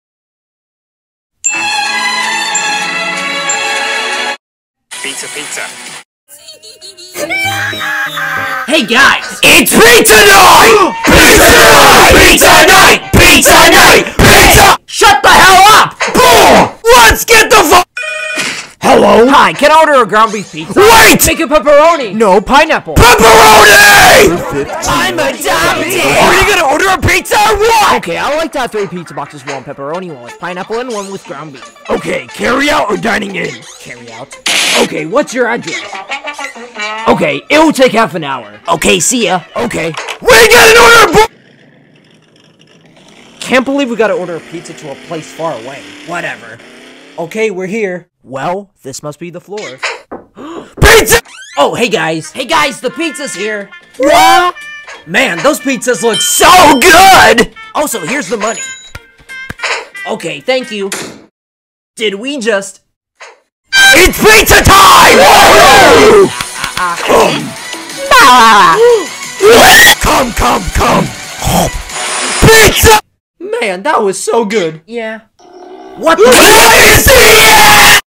Dillard And The Insane World! E3 S2: Pizza Night!
Today was the night of their lives PIZZA NIGHT! Dillard and friends goes and orders some delicious pizza to go crazy with!

The footage in the beginning belongs to @Lil-coast !

Special thanks to: @Alex.Fernandezzz and @Bird0Animations for helping me with animating! :)